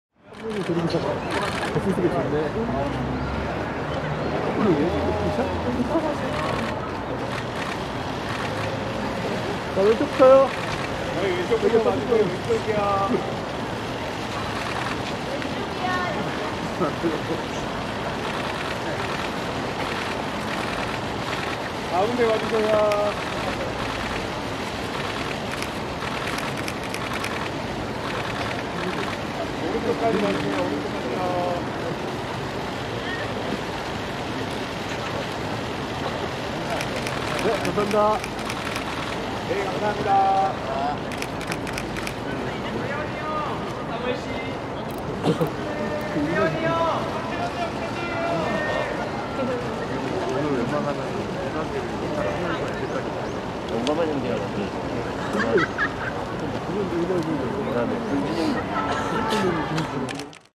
용감한 홍차 with사무엘 (김사무엘, Samuel) @ 190222 뮤직뱅크 출근길
2월 22일 오전 KBS 뮤직뱅크에 김사무엘,차태현,홍경민이 리허설 녹화에 참여하기 위해 출근하고 있다.